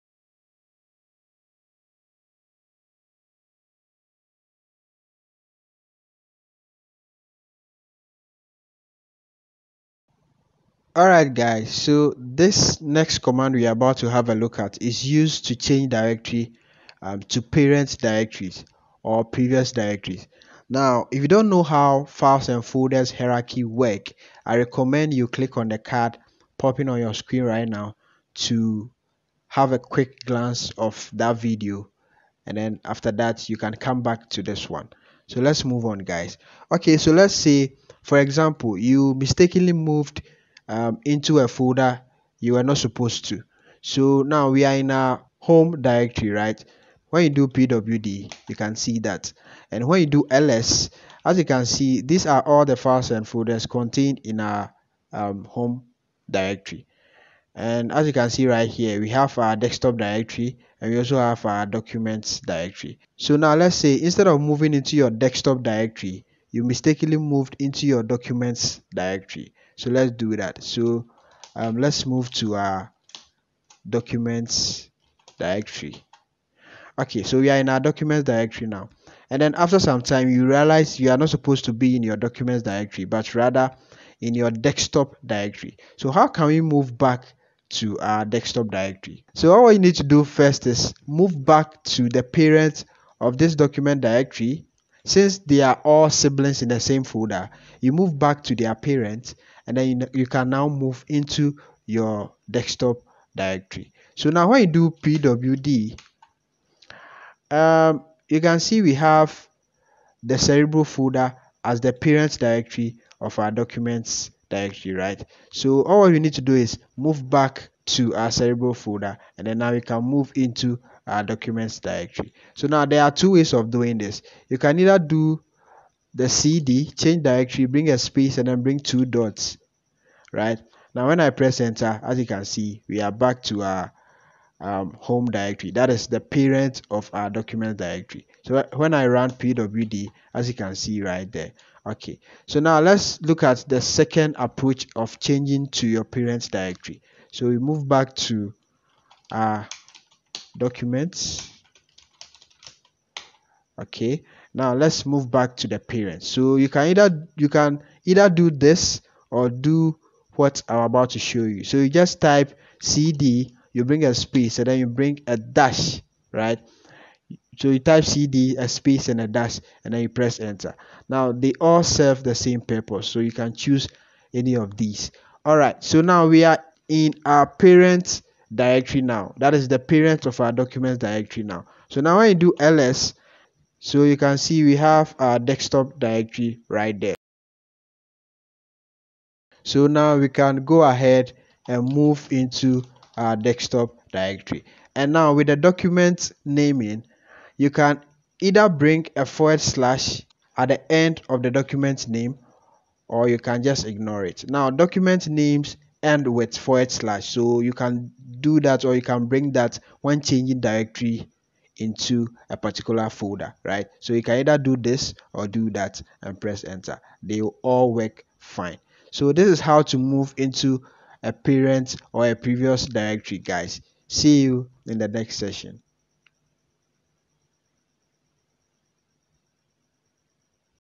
Introduction to gitbash | cd .. and cd - #08
In this tutorial, we are going to look at how we can change directory to parent folders or previous folders using a special form of the cd command.

If you don't know how files and folders hierarchy work, i recommend you check out the link below before you come back to this video